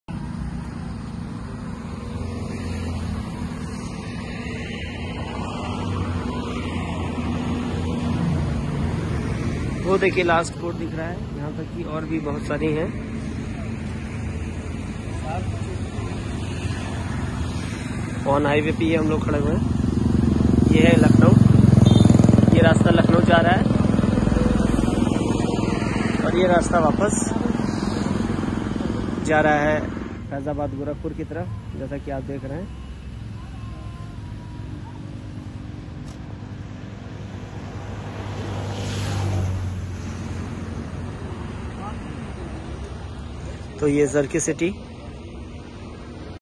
Elpida Homes Zharki City By Ghardhudho.com, For Site Visit
Elpida Homes presents Zharki City : A Free Hold Plot In Lucknow. This Project is Marketed and Promoted by Ghar Dhudho Dot Com.